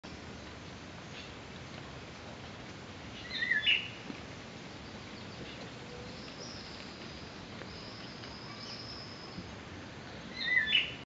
parasolmieren
parasolmieren kunnen in één dag een hele boom leeg plukken, er zit dan geen blad meer aan de boom. De stukjes blad worden meegenomen naar het nest waar dan weer schimmels op gekweekt worden die tot voedsel dienen van de mieren